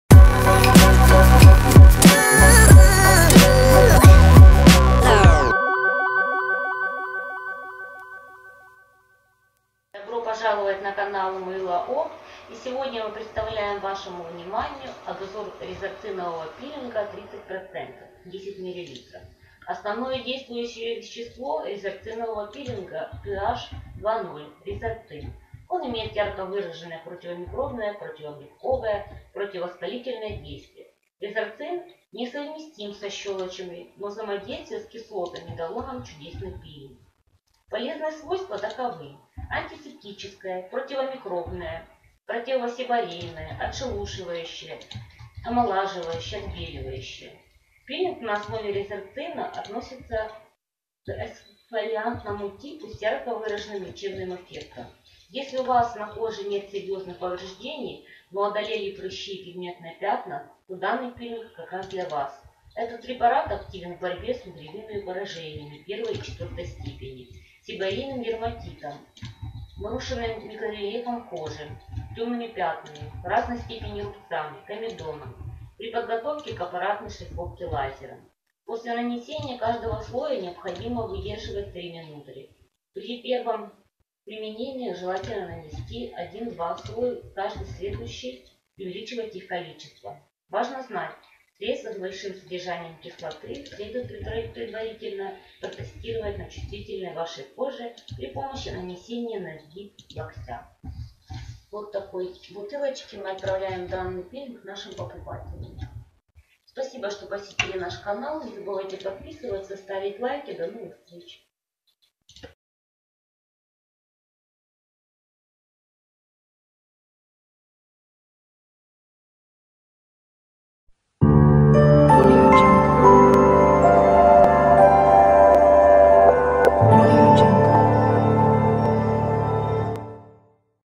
Резорциновый пилинг 30% рН 2.0 // Пилинг для лица // Салонная косметика дома
Видео обзор Резорциновый пилинг 30% рН 2.0 от магазина Мыло-опт